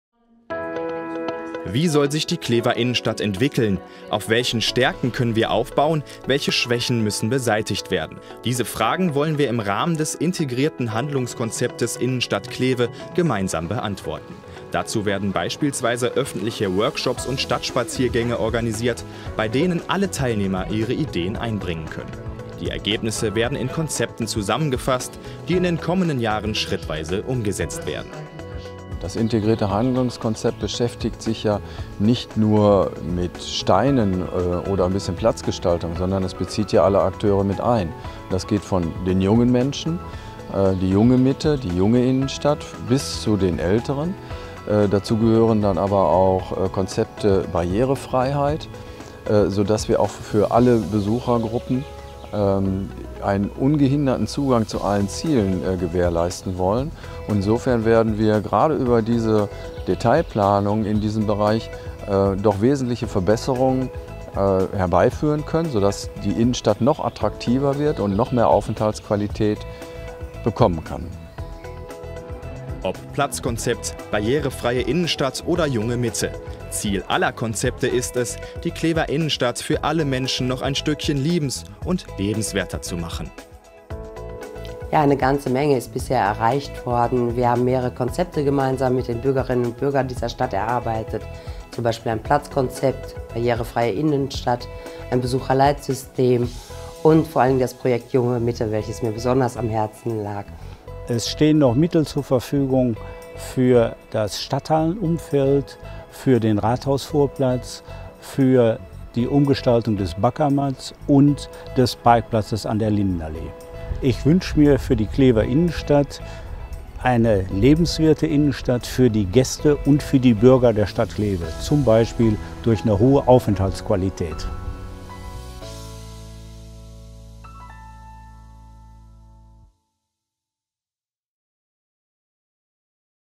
Entwicklung der Klever Innenstadt
Wie soll sich die Klever Innenstadt entwickeln, auf welchen Stärken können wir aufbauen und welche Schwächen müssen beseitigt werden!